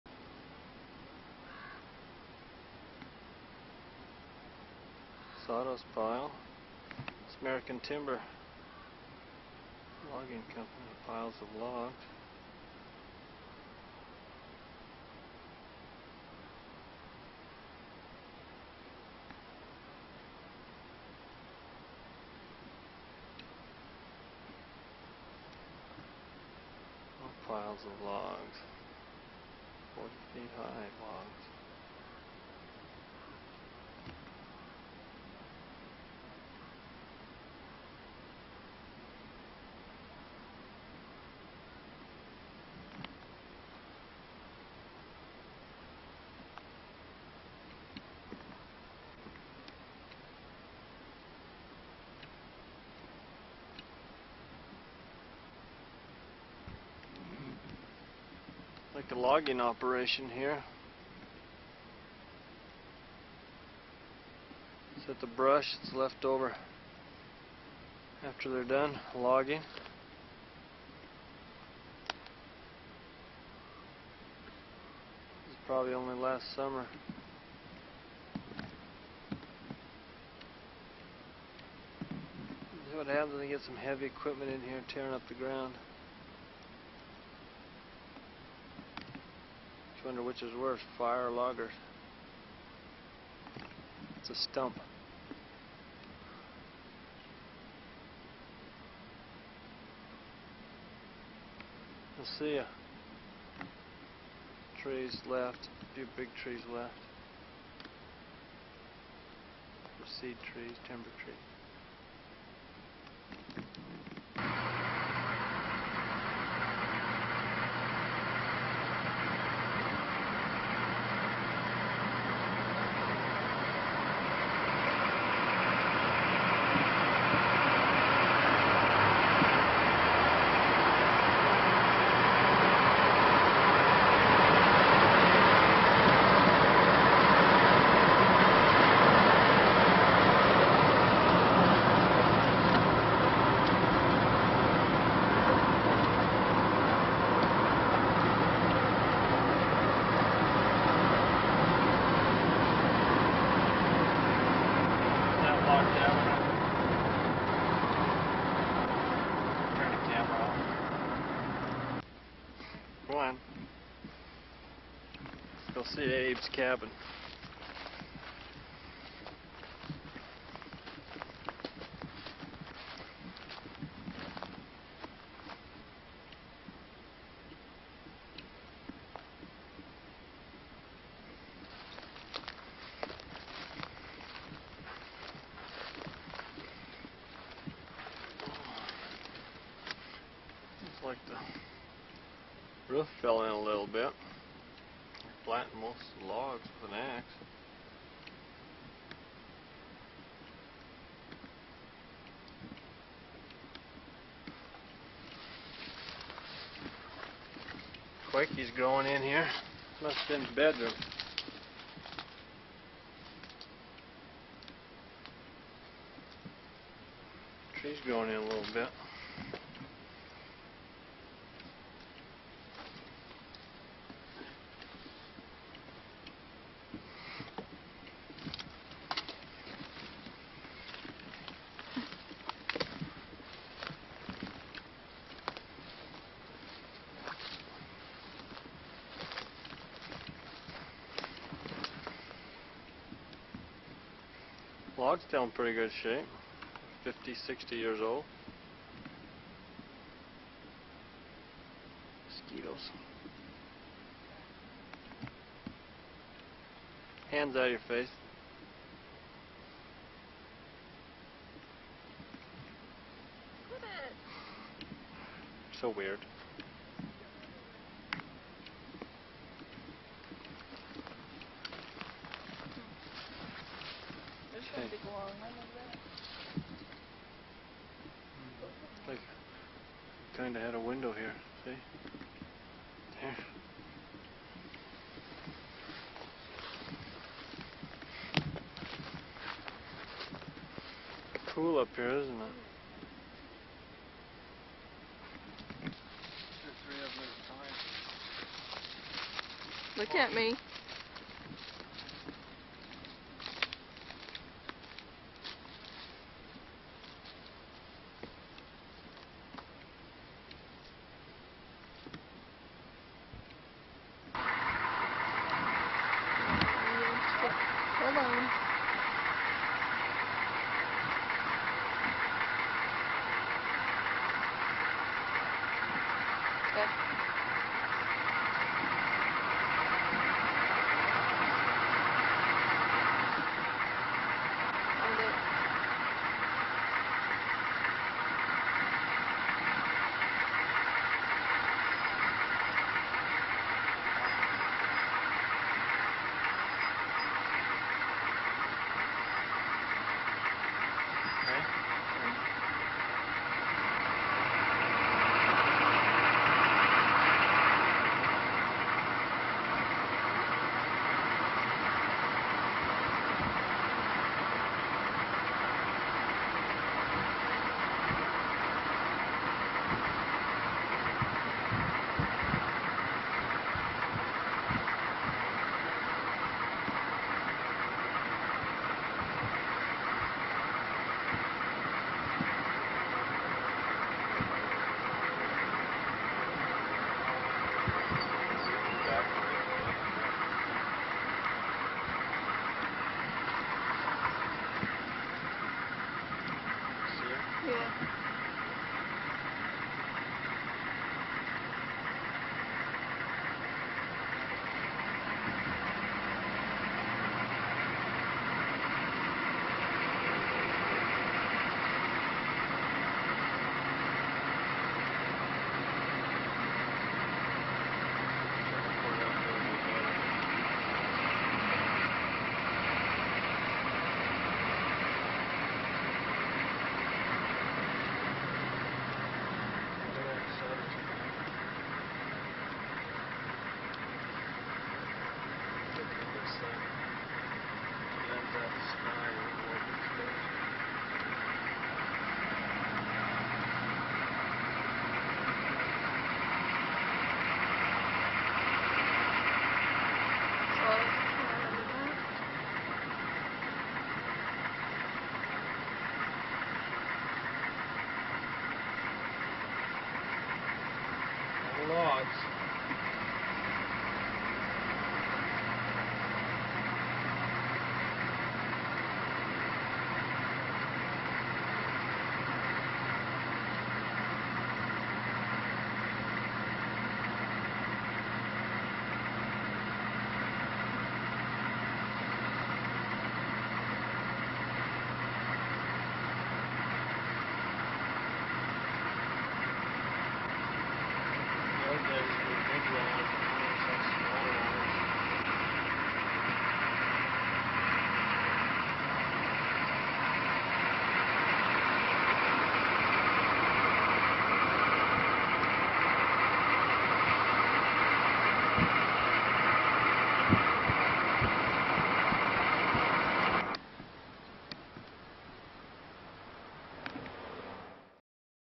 Lodge Pole Logs Biomass all american timber
Jack Pine Lodgepole Pine, geting out the timber befor winter shuts you out till next June. As grampa would say " you got a jag of timber there" he logged with mules in 1920's.